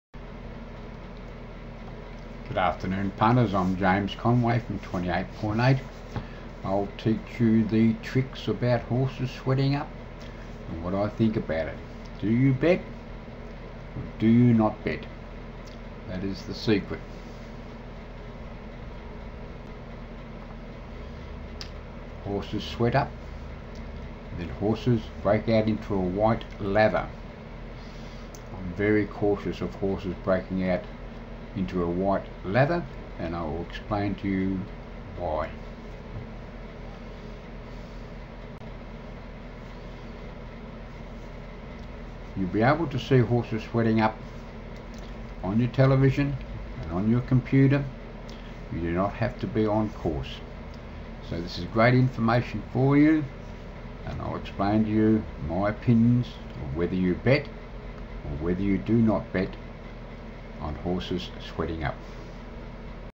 28.8 Promo Video Horses Sweating Up Bet or No Bet
Promo Video 28.8 Secrets to Horses Sweating Up, Bet or No Bet.
World-wide release of never seen before 18 Horse Fitness Videos, How 2 Rate a Horse's Blood count + more OUT MONDAY 25th August. A huge edge for punters and trainers.